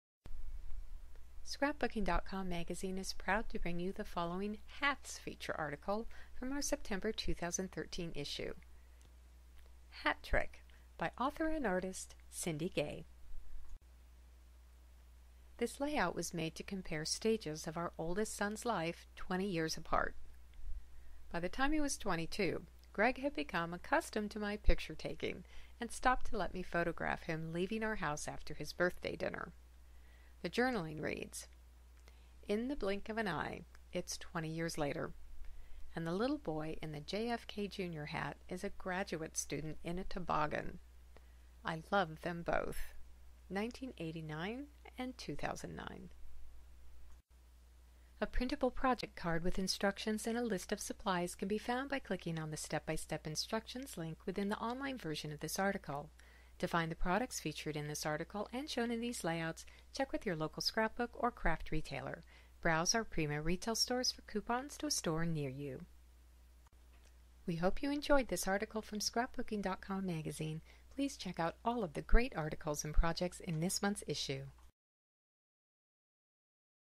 Hats Article - September 2013, Scrapbooking.com
-- A monochromatic color scheme gives this page a masculine look.